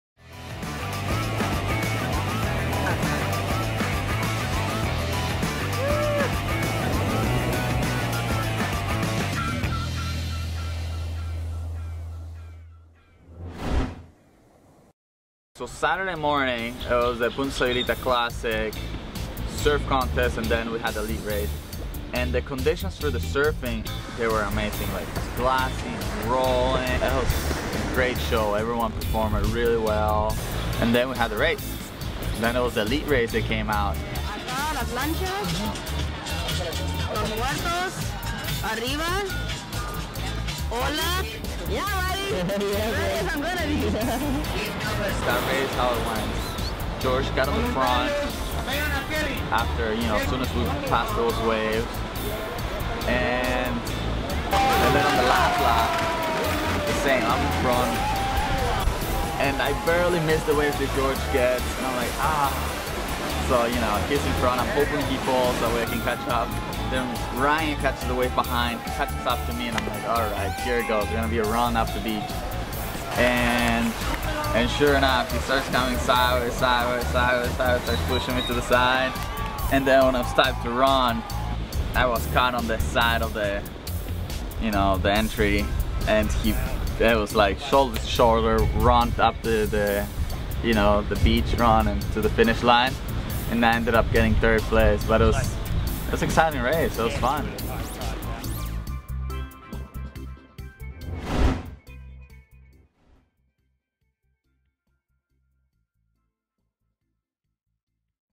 Purakai TV with Fernando Stalla Episode #3: The Paddle Race
In this episode Purakai Ambassador Fernando Stalla recaps his 3rd place finish in the long distance paddle race of the Punta Sayulita Longboard & SUP Surf Classic 2014 Sayulita, Nayarit, Mexico.